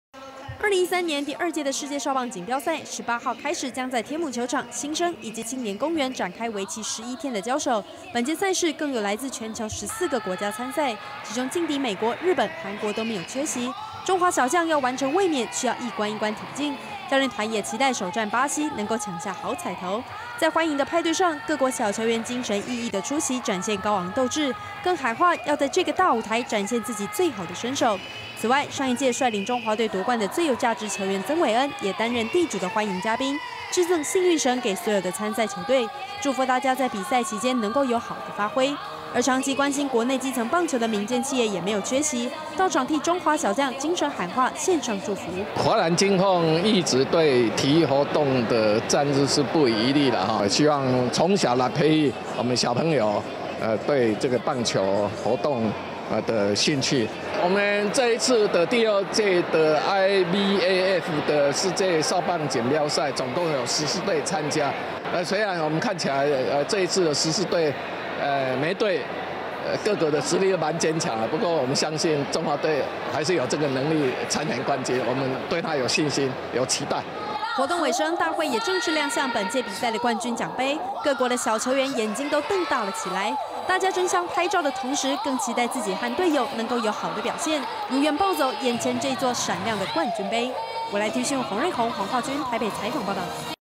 【 IBAF 】14國好手過招，中華小將力拼衛冕
2013年IBAF世界少棒錦標賽即將開打，本屆共有14個國家參賽，激戰程度精采可期，17號賽前歡迎派對小球員們盛裝出席，中華隊更喊話要完成衛冕，台北市長郝龍斌也表示很開心再度承辦這項國際賽事，期待所有小朋友享受比賽的同時，更能彼此交流當朋友。